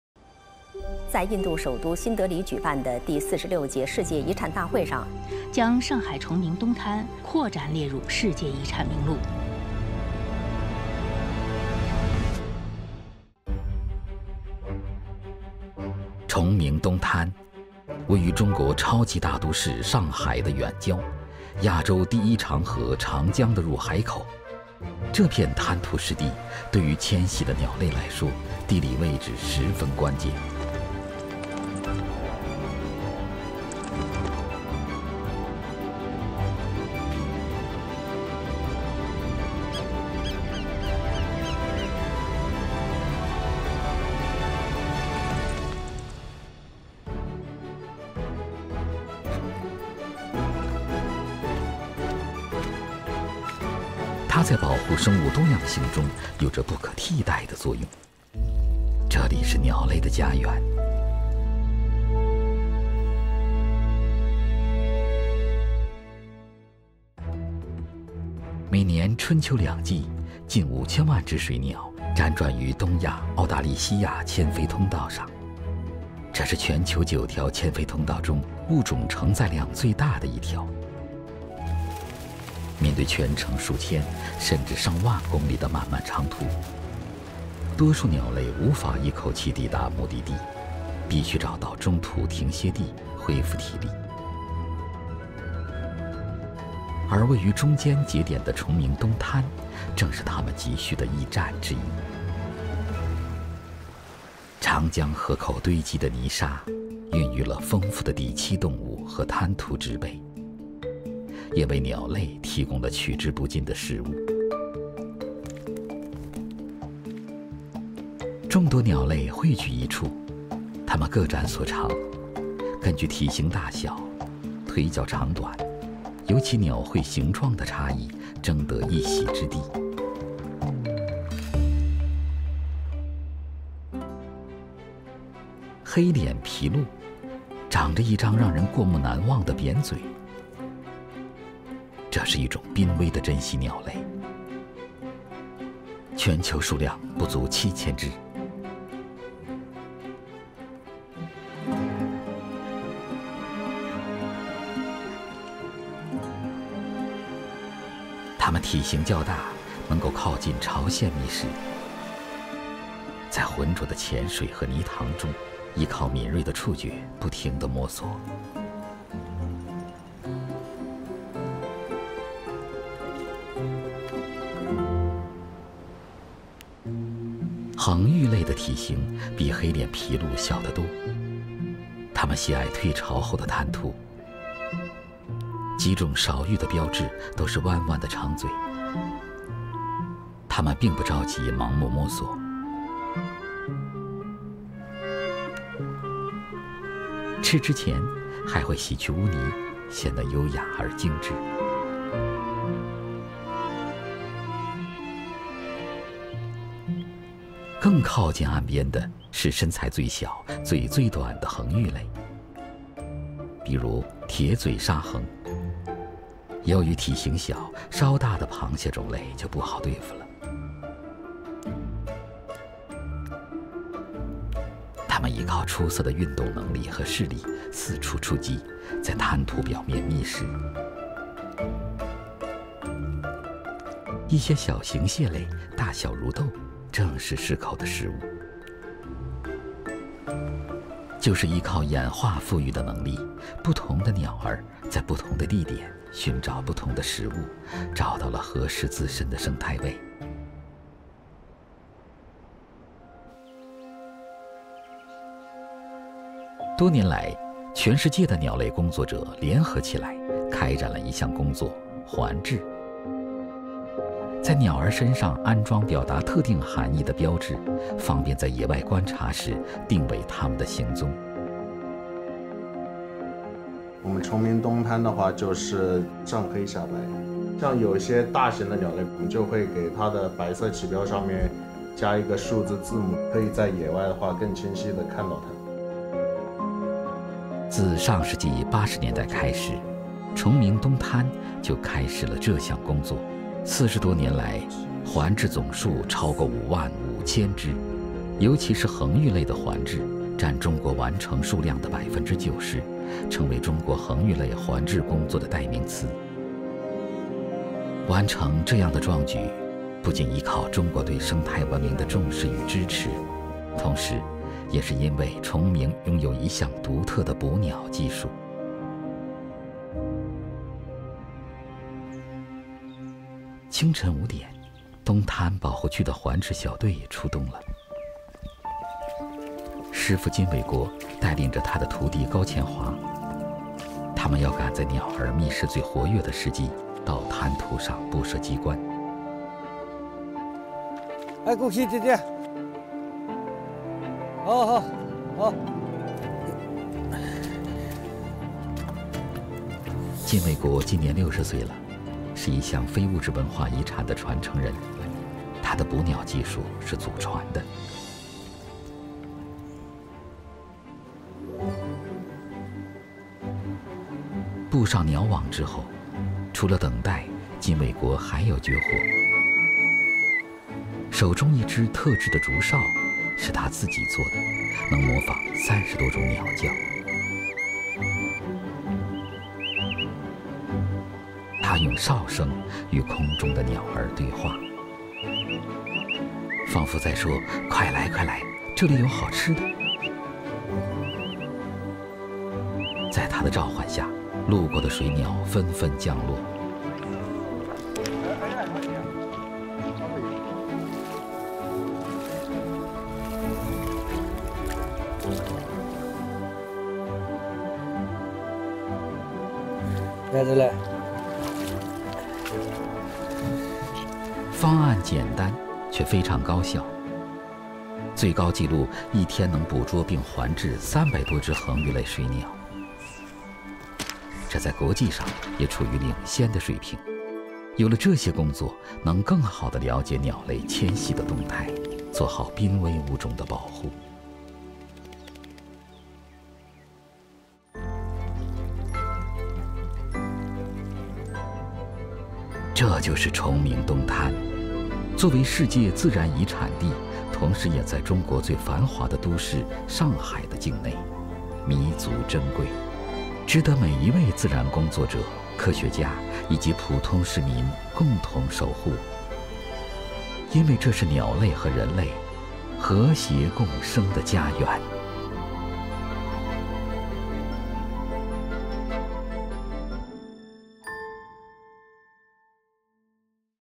东滩，鸟类的家园
8分钟带您速览 上海 首个世界 自然遗产，近距离了解候鸟和人类相处的有趣故事。Take a fast 8-minute tour of Shanghai's first World Natural Heritage Site and learn about the fascinating stories of migratory birds and people.